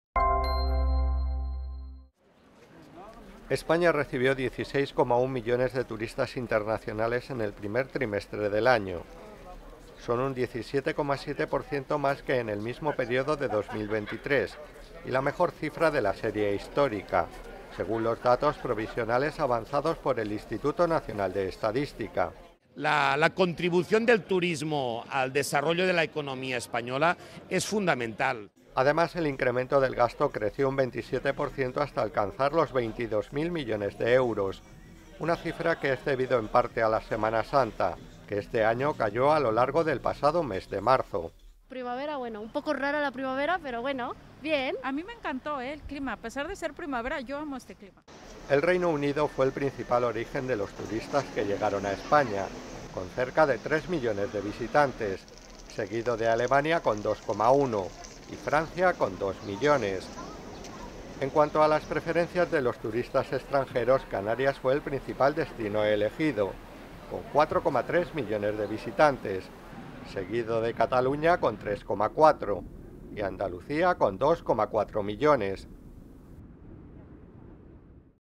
El turismo cierra el mejor primer trimestre de su historia con 16,1 m. de visitantes
Madrid, 3 may (EFE).- El número de turistas internacionales que visitó España superó los 16,1 millones hasta marzo, tras registrar un incremento del 17,7 %, mientras que su gasto creció un 27,2 % hasta los 21.948 millones de euros, en el mejor primer trimestre de toda la serie histórica, que este año ha incluido la Semana Santa.
Edición y locución: José Antonio Gallego.